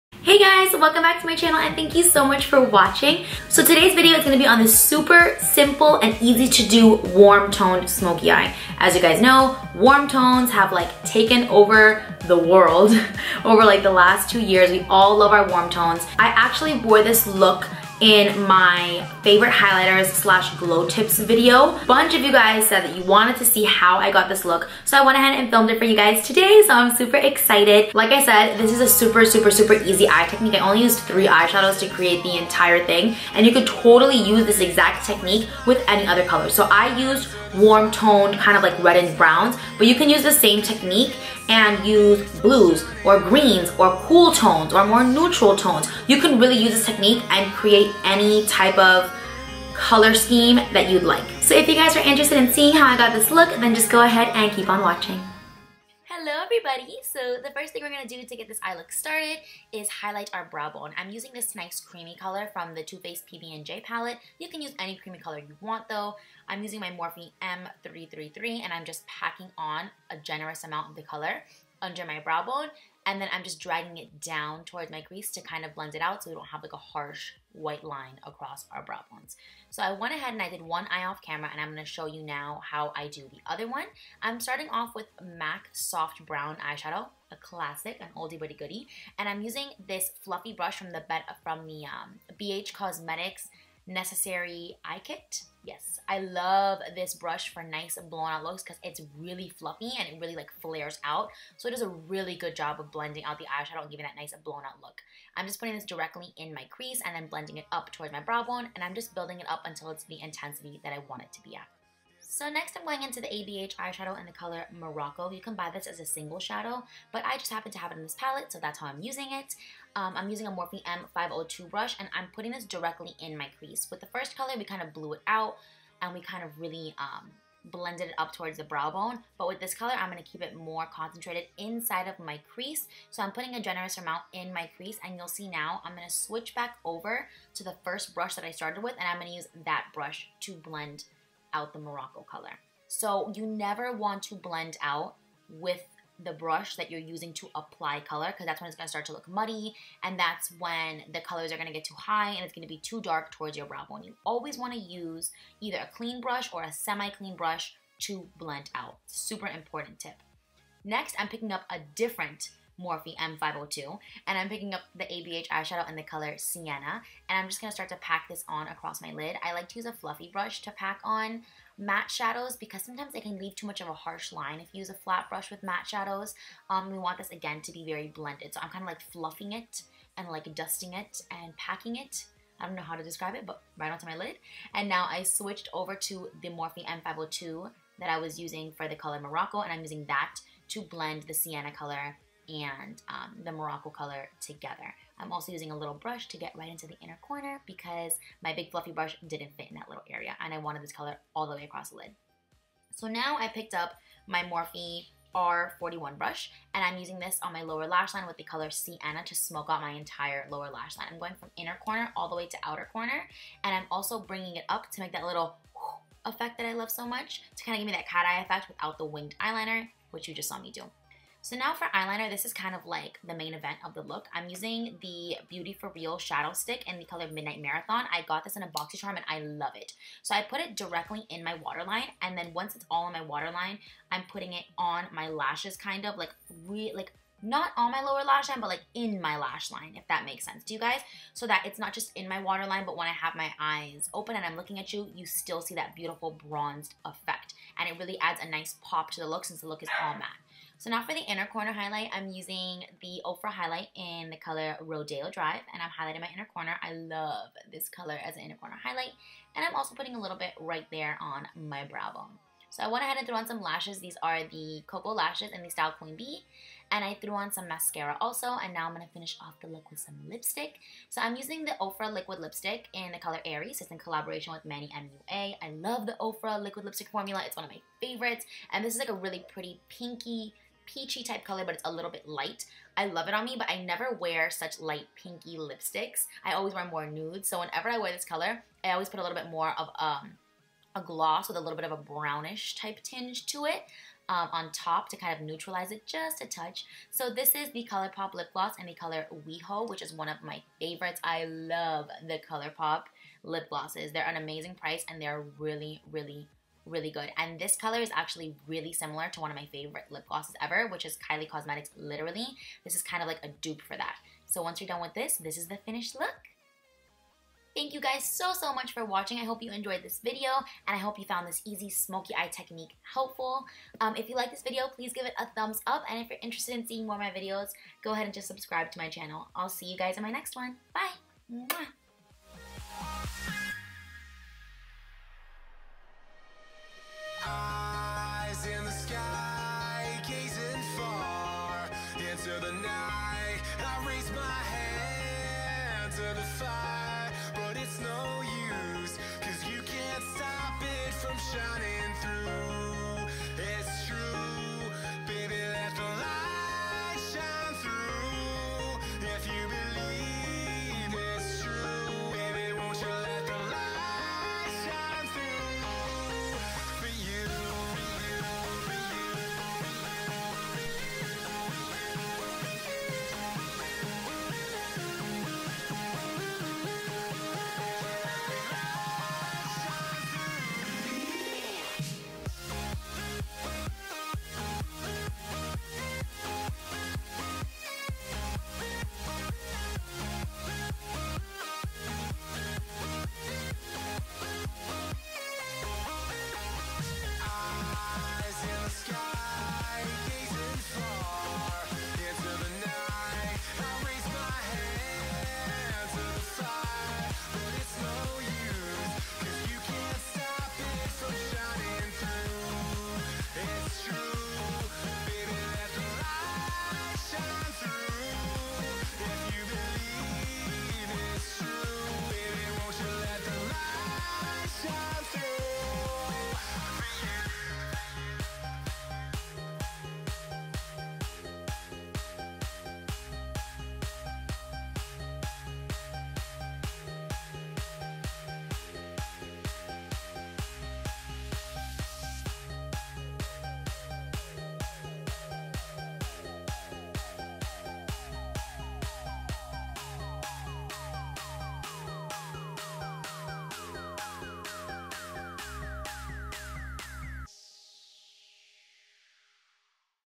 EASY WARM TONED SMOKEY EYE | SUPER SIMPLE EYE TECHNIQUE | ABH & MUG
Hello, Beauties!! Hope you enjoyed this video and found this technique helpful! I use this technique A LOT when I'm in a rush but still want to have a soft smokey effect to my eyes. Thanks again for watching!

PRODUCTS USED:

MAC "soft brown" eyeshadow
ABH "morocco" and "sienna" eyeshadows
Beauty For Real shadow stick in "midnight marathon"
Koko lashes in style "queen bee"
Ofra Cosmetics liquid lip in "aries"
Colourpop gloss in "weho"